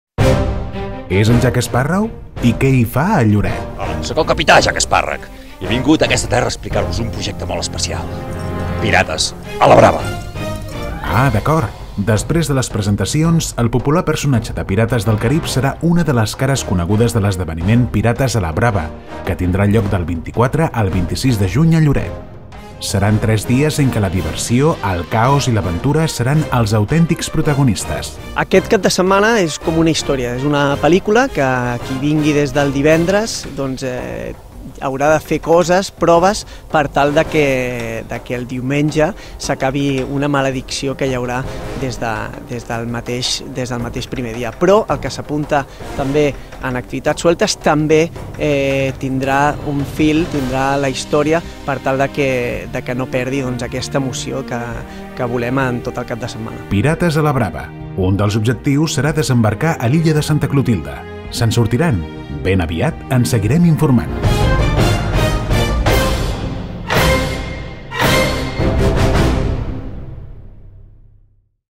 Què fa Jack Sparrow a Lloret?
El popular personatge serà una de les cares conegudes de l'esdeveniment "Pirates a la Brava", que es durà a terme del 24 al 26 de juny a Lloret.